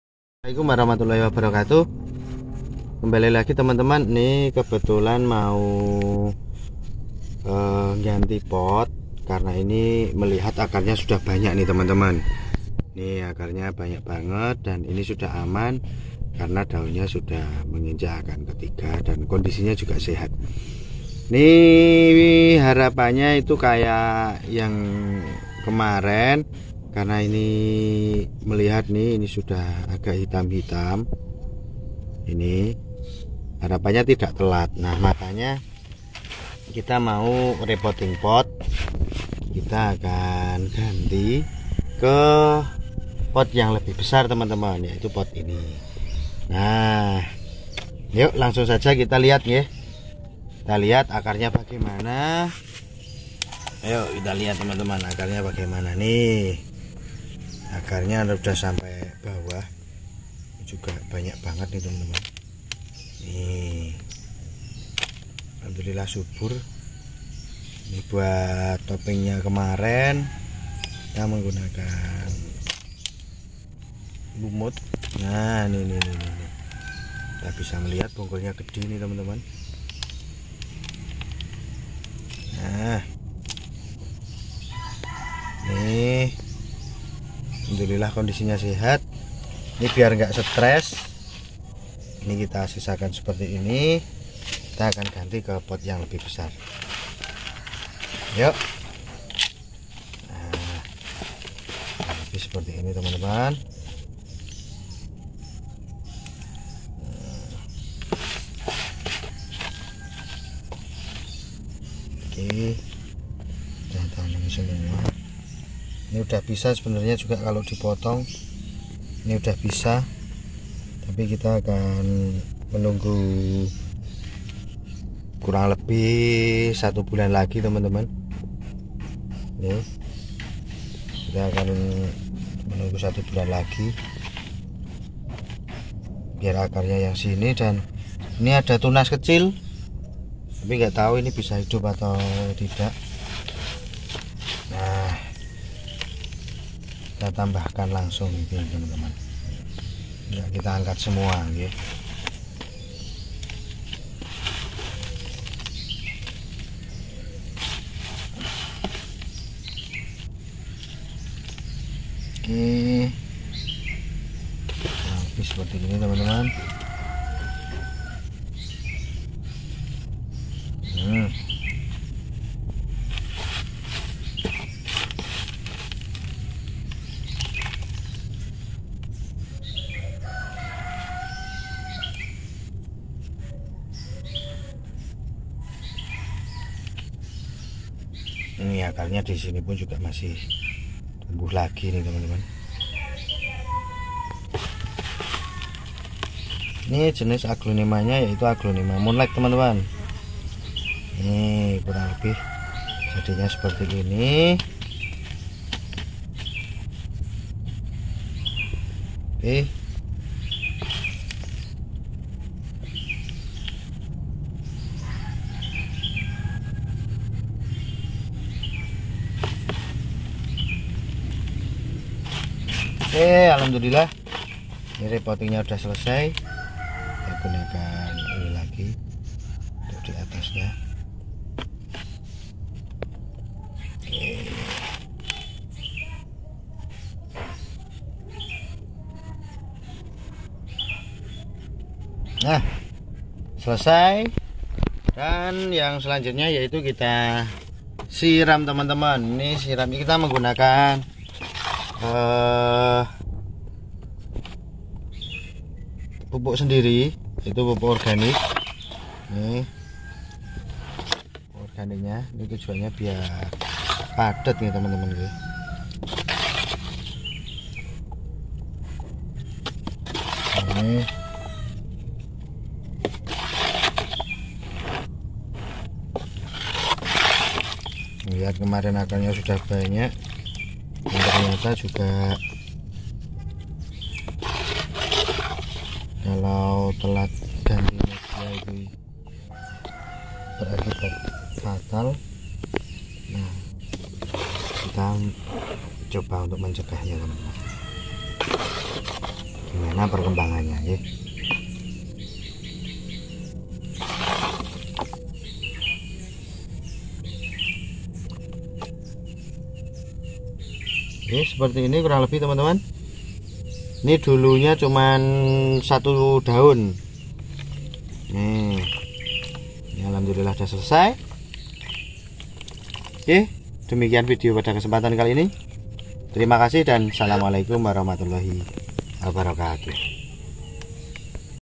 aglonema moonlight ganti media sebelum terlambat
Dalam memelihara taneman media sebaiknya di ganti secara rutin jangan sampai terlambat karena menjadikan media lembab menimbulkan tempat singgah bakteri